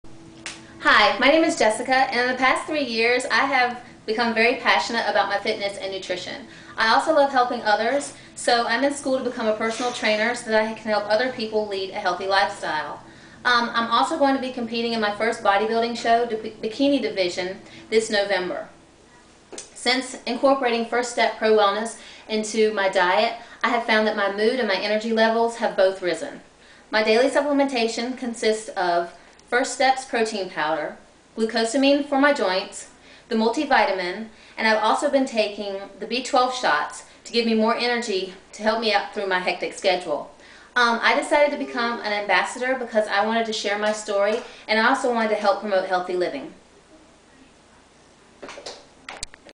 Meet PRO-WELLNESS Ambassador Jessica!
Jessica is a wife and mother of three girls. For the past two years, she has been gradually making the lifestyle changes of working out and eating "clean". It is a decision she wished she'd made long ago! She is so passionate about her decision to live healthily that she decided to make it her profession and is now in the final stages of becoming an ISSA Certified Personal Trainer. Jessica is also training for her first professional bodybuilding competition, the NPC North Carolina Elite.